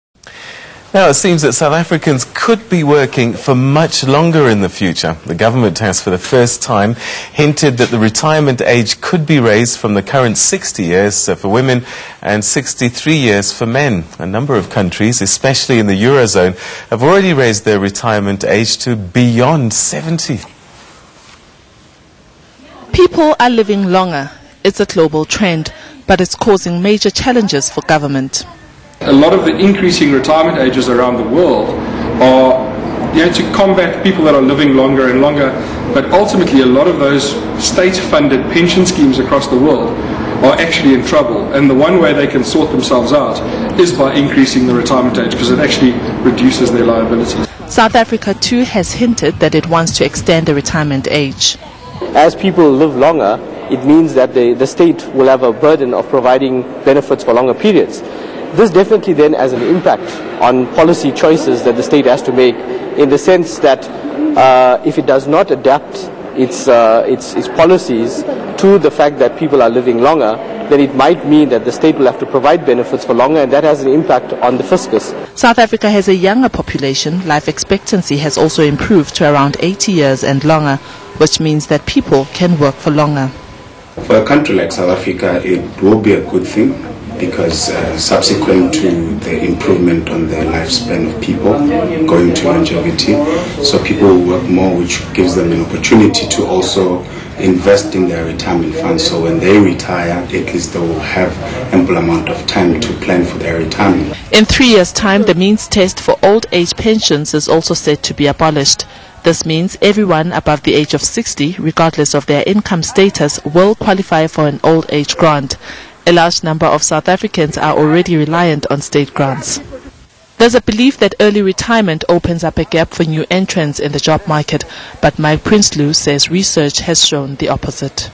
2013 06 21 SABC2 Alex Forbes 07h15
Mike Prinsloo from Alexander Forbes adds to the conversation on SA increasing the age for retirement.